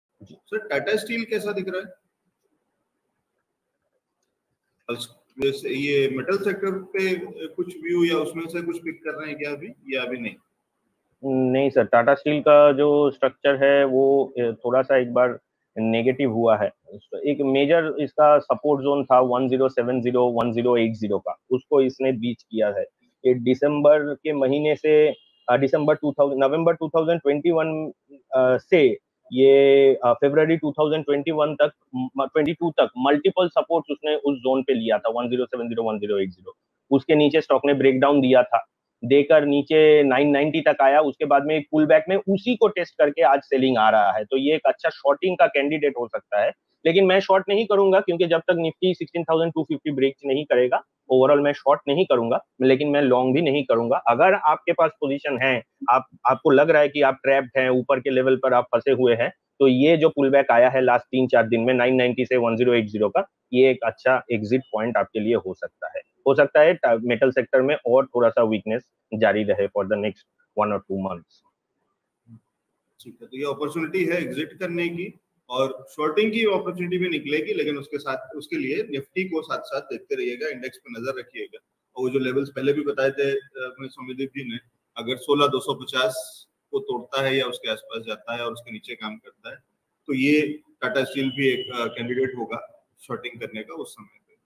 Tata Steel Limited के शेयर में क्या करें? Expert Opinion by Soumadeep Bhattacharjee
जानिए हमारे stock market expert Soumadeep Bhattacharjee की राय |

Analysis of: Tata Steel Limited